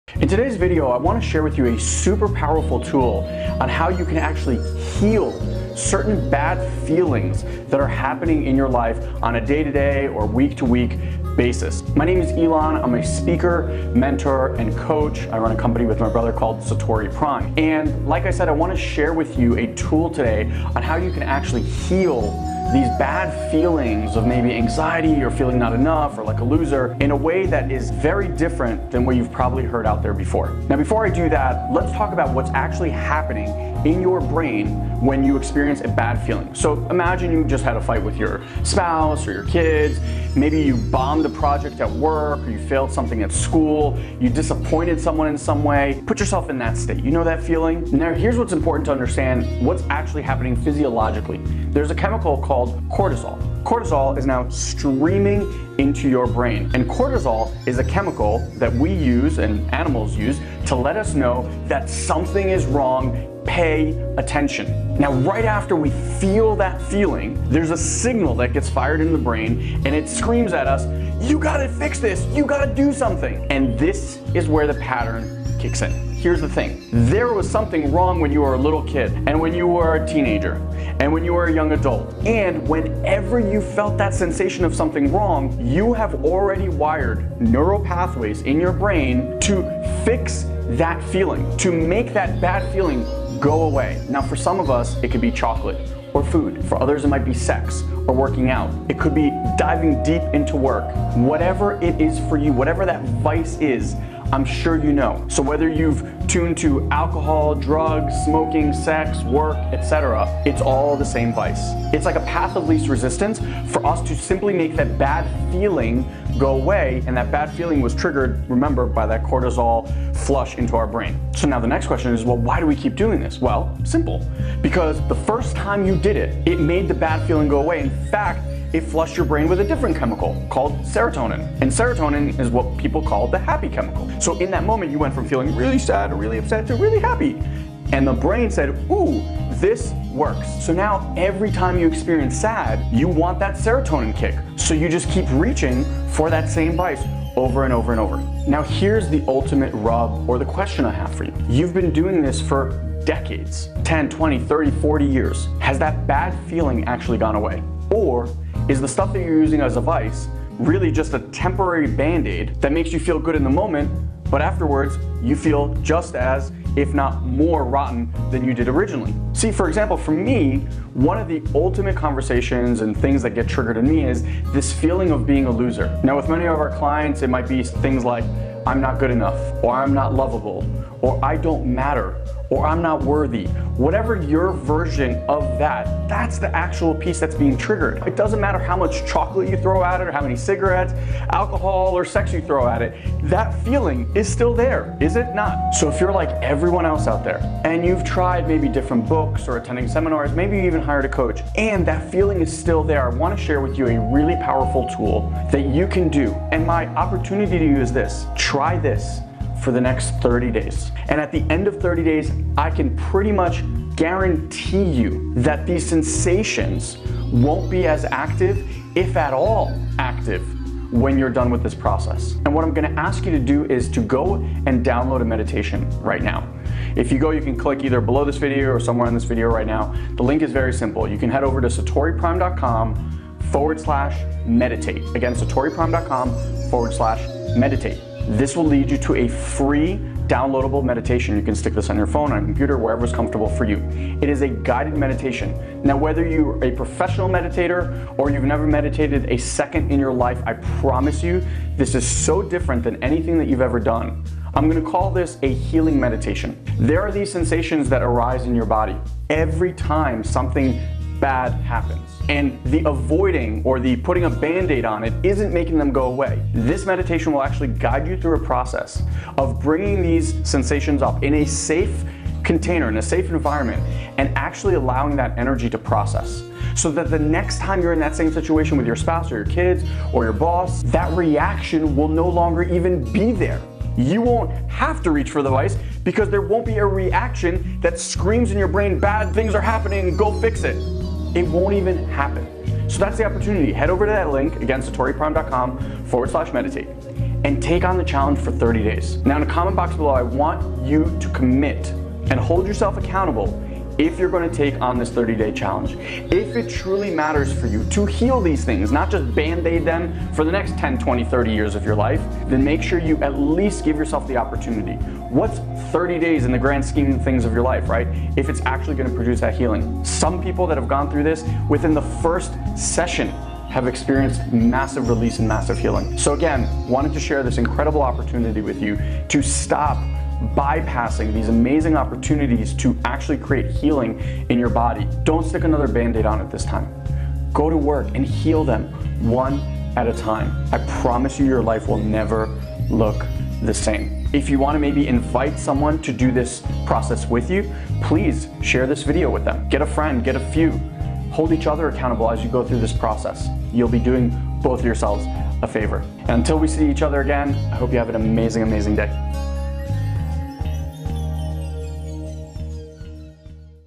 How to Stop Feeling Stressed, Depressed and Anxious all the Time
Learn how to stop feeling stressed out, depressed and anxious with Ilan Ferdman. Today Ilan shares a super powerful tool to heal certain bad feelings  that are happening in your life.